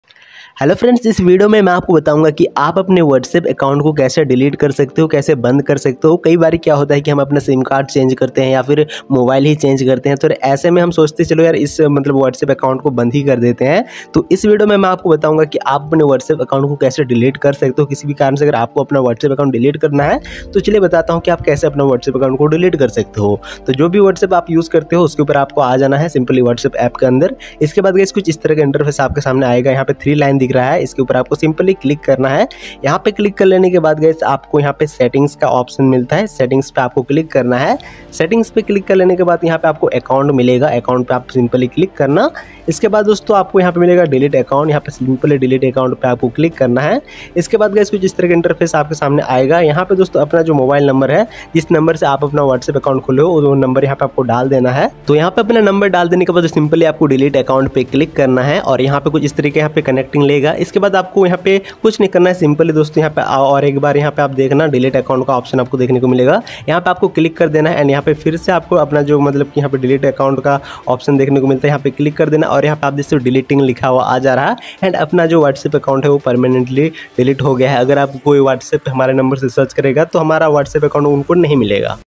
Whatsapp account delete | Whatsapp account kaise delete kare | How To Deactivate Whatsapp Account
Hello Friends,
In This video I will tell u That How To Delete Whatsapp Account Permanently or whatsapp account delete kaise karte hain Explained in Hindi.

If u like the video Then